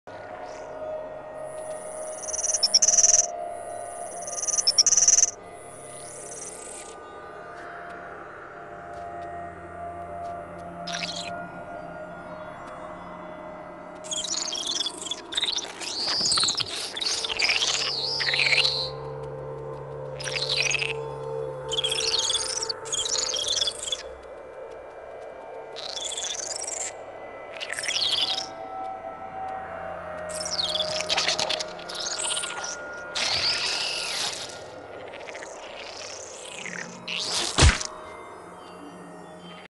Spidey
First attempt at making a spider type creature